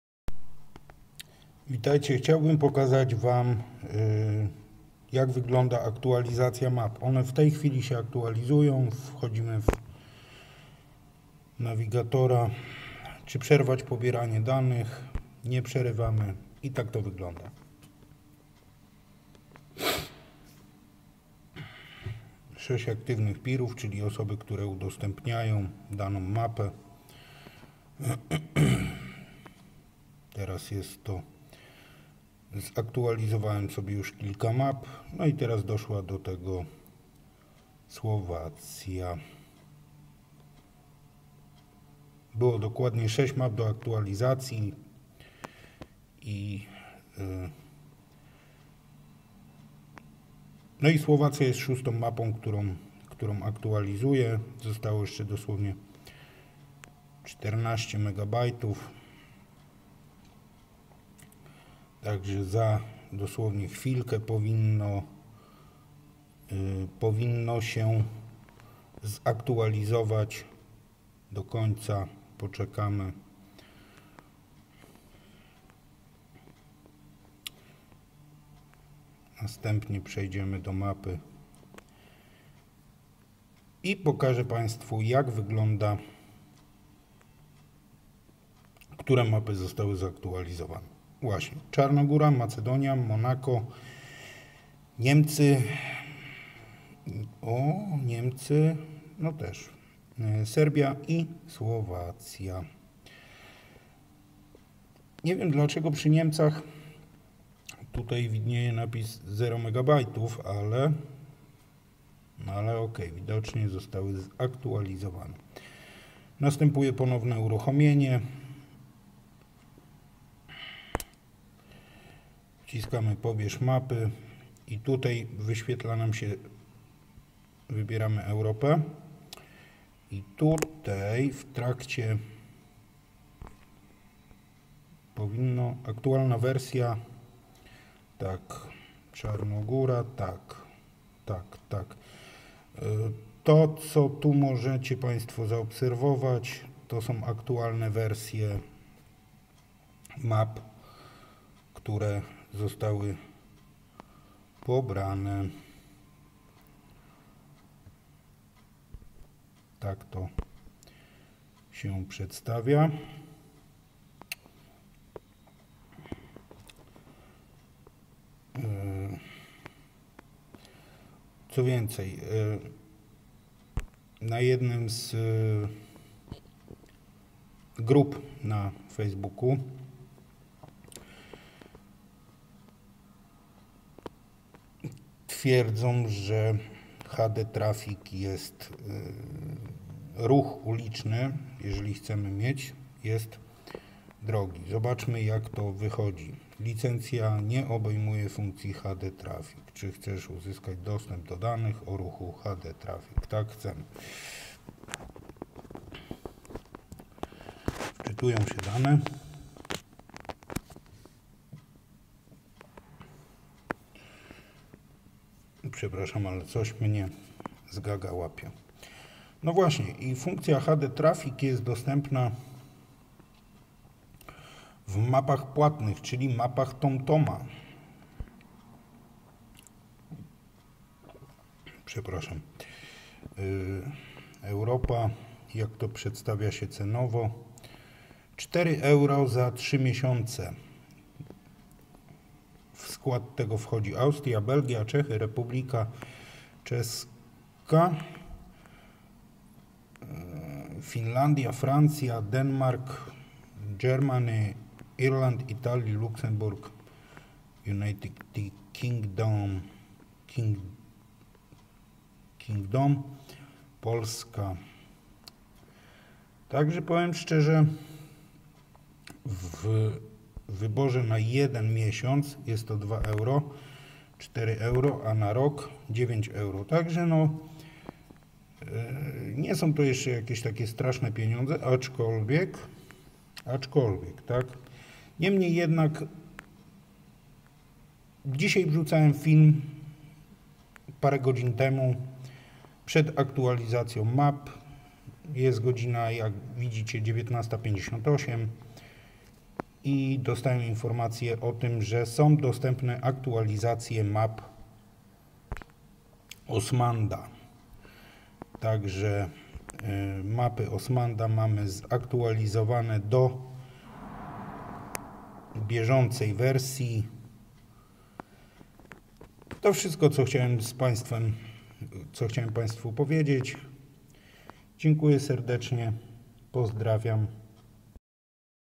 Aktualizacja map OSM Navigator
Krótki film instruktażowy odnośnie aktualizacji map OSM nawigator